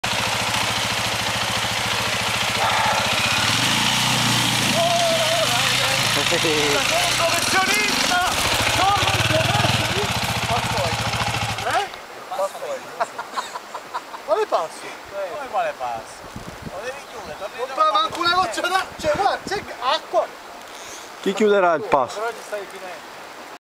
MVI_0183 Guado Professionista.AVI
Alberto ci spiega come attraversare con pochi schizzi. E' più bravo a raccontare barzellette!!!